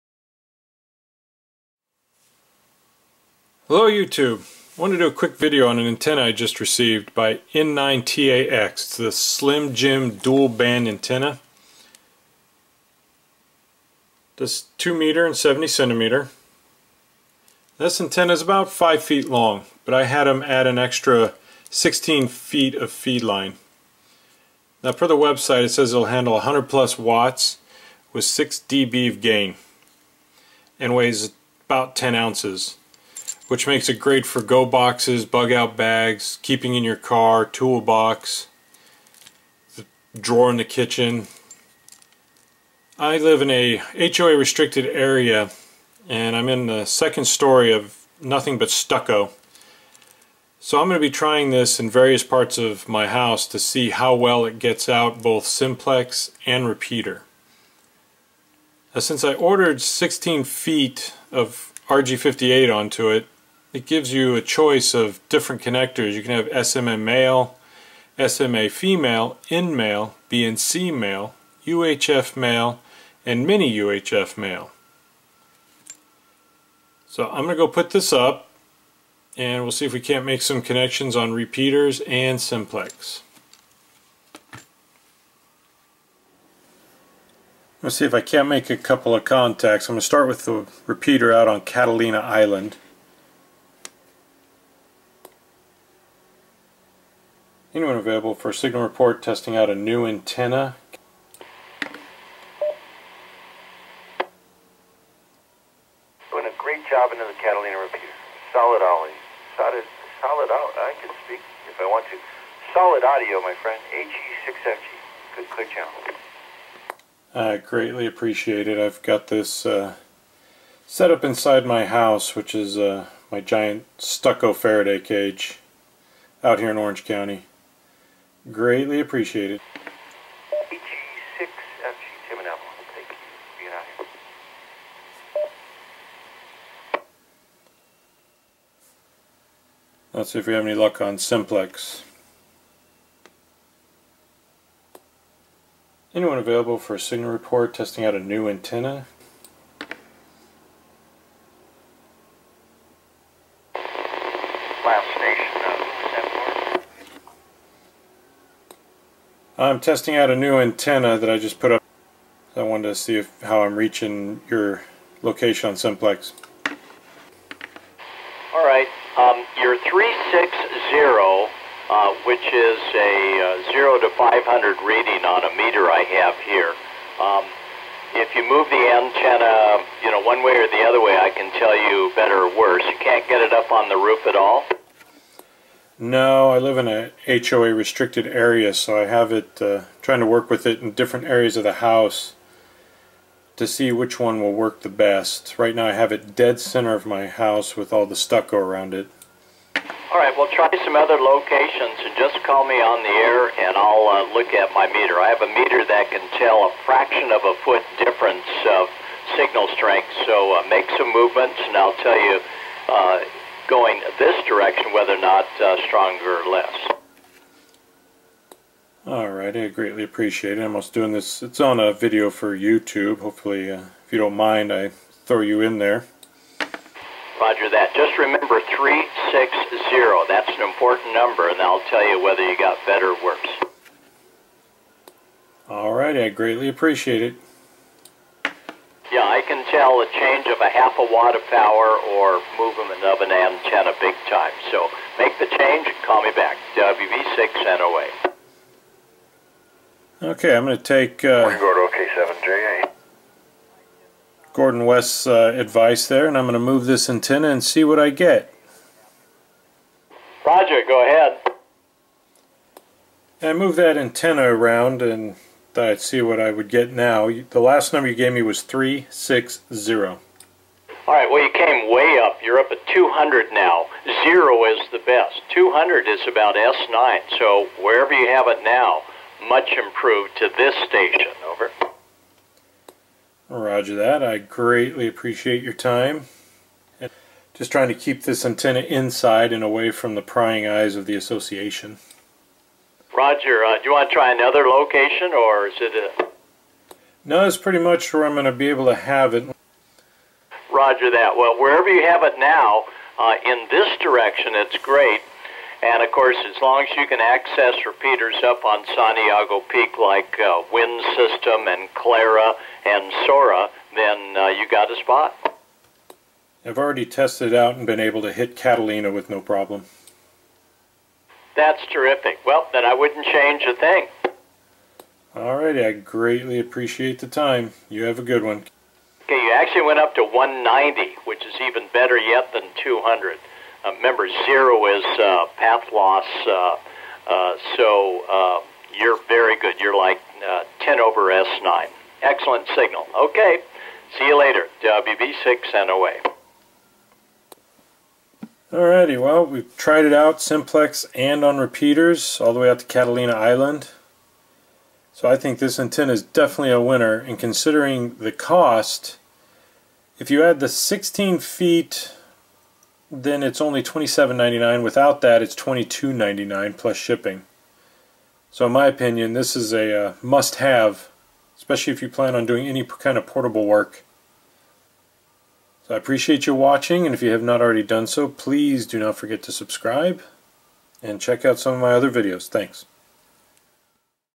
Ham Radio Antenna - N9TAX Slim Jim Dual Band Antenna Review
Check out this AMATEUR RADIO VIDEO!
In this video, I show you the N9TAX Slim Jim Dual Band Antenna 2m/70cm